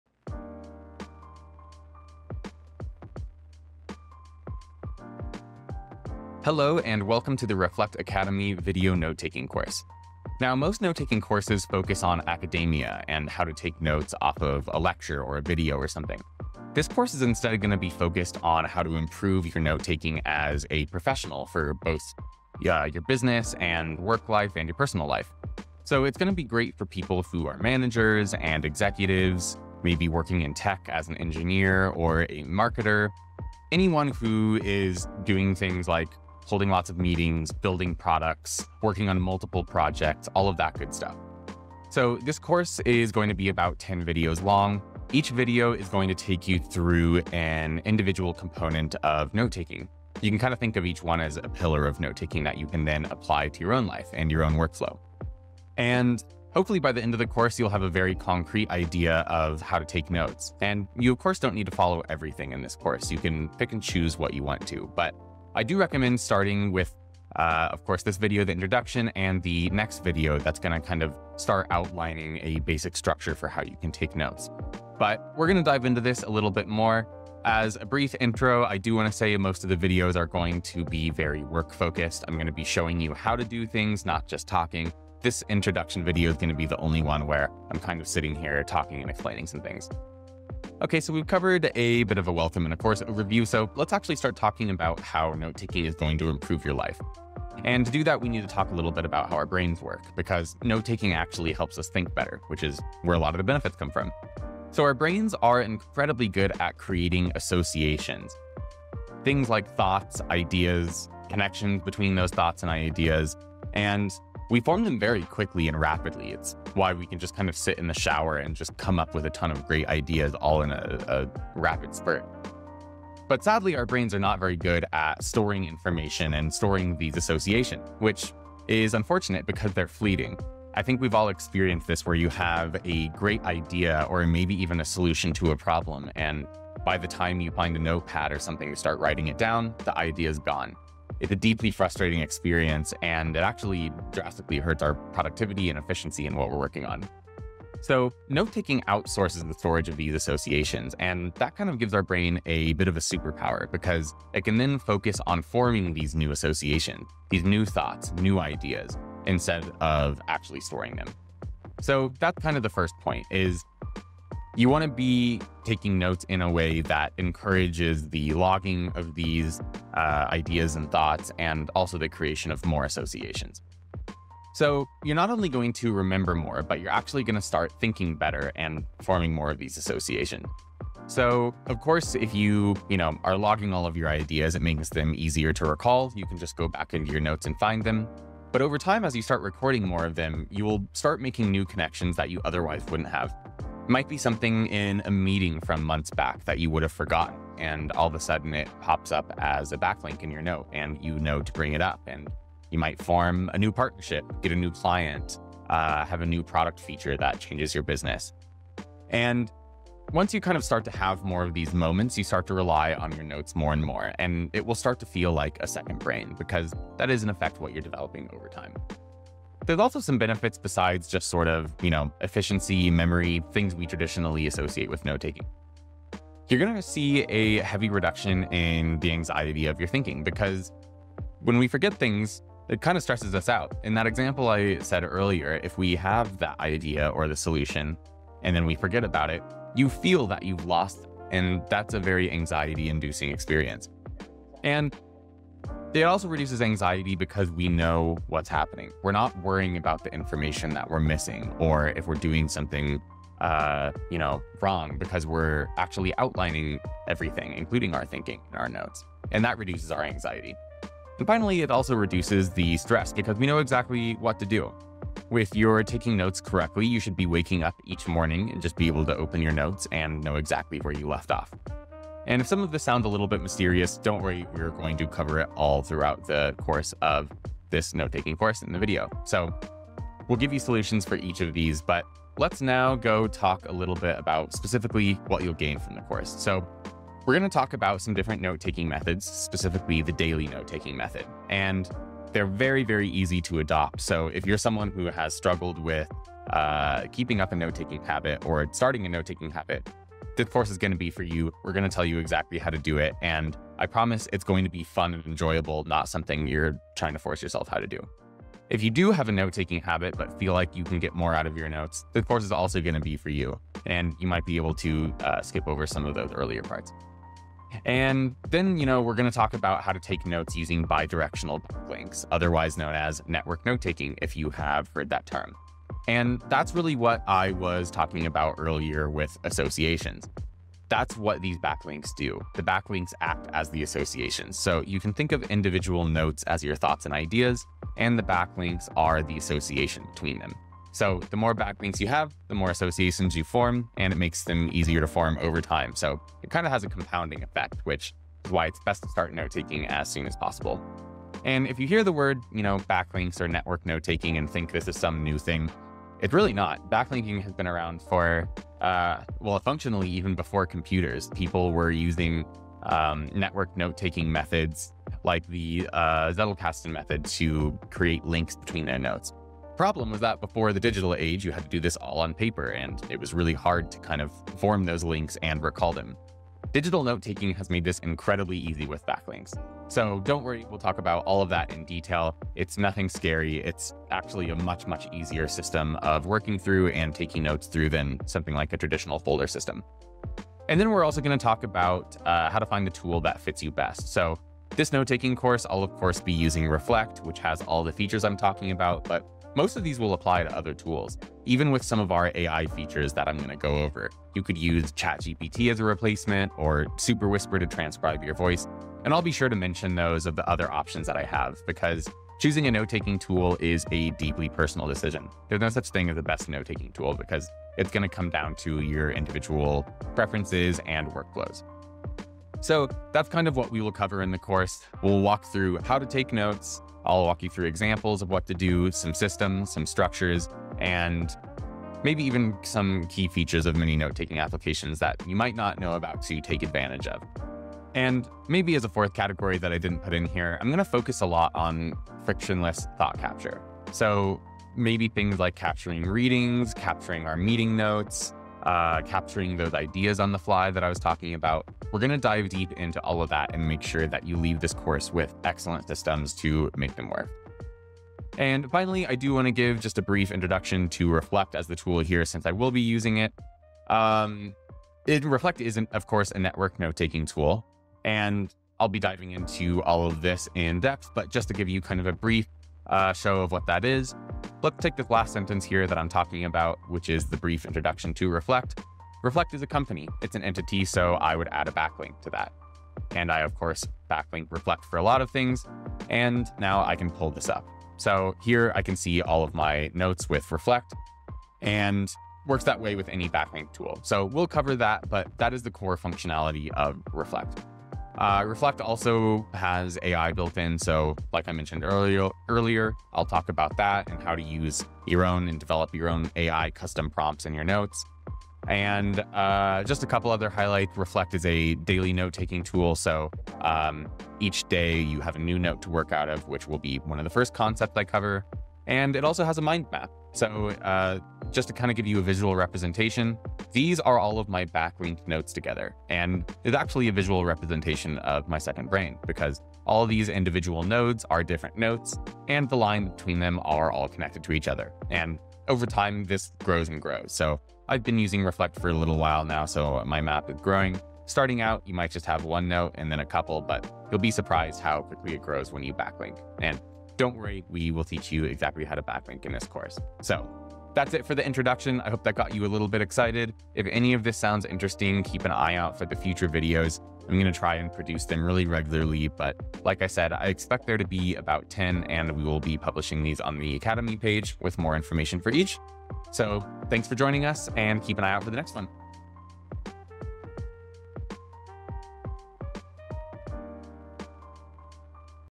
Reflect Academy: Intro to Note-taking
Welcome to the Reflect Academy video series – an easy to follow note-taking course that teaches you how to take simple and effective notes. If you're looking to learn how to take better notes, this is the place!

The series will be around 10 videos long with each one lasting ~10min. In each you'll learn a fundamental concept to modern note-taking. Vidoes will be published here and on reflect.academy.

The note-taking tool used for this series is reflect.app.

This first video is an introduction to the course and note-taking more broadly.  Up next, we'll talk about the 5 Principles of Note-taking.

Chapters
00:00:00 - Introduction to the Course
00:00:19 - Target Audience & Course Length
00:00:38 - Components & Structure of Course
00:00:56 - Starting Recommendations
00:01:16 - Course Objectives
00:01:31 - The Importance of Note-Taking
00:01:50 - How Our Brains Work
00:03:22 - Benefits Beyond Efficiency
00:04:40 - What You’ll Gain
00:05:19 - Bi-Directional Backlinks & Network Note-Taking
00:06:16 - Choosing Your Note-Taking Tool
00:07:17 - Frictionless Thought Capture
00:07:37 - Introduction to Reflect as a Tool
00:09:00 - Conclusion & Next Steps